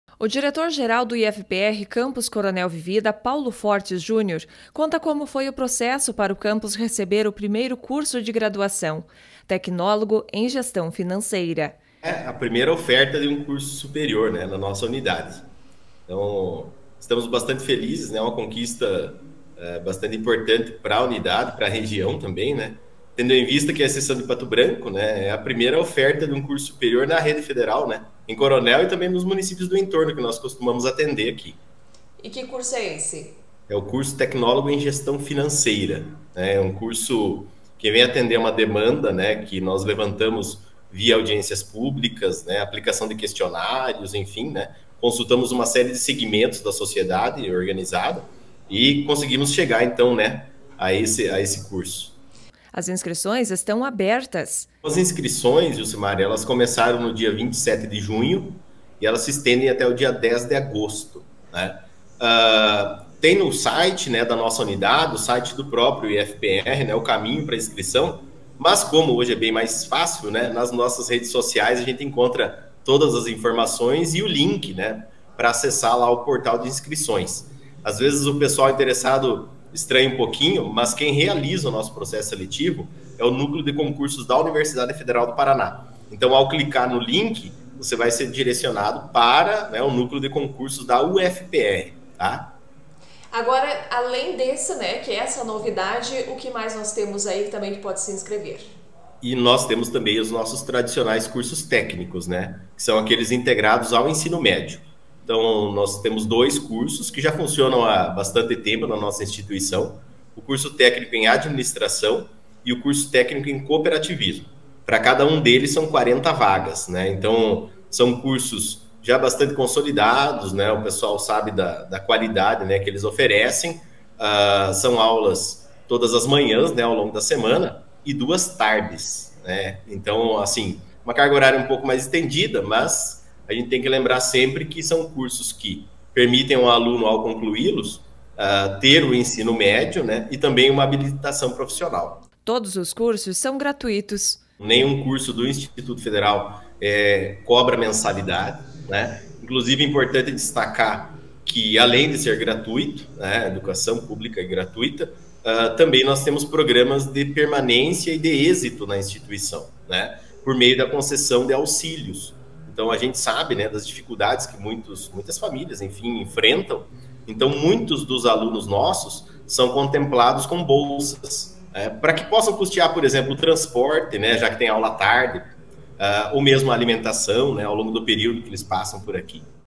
IFPR Campus Coronel Vivida está com Processo Seletivo aberto
Instituto Federal do Paraná Campus Coronel Vivida, terá um curso superior na Unidade. Processo Seletivo está aberto.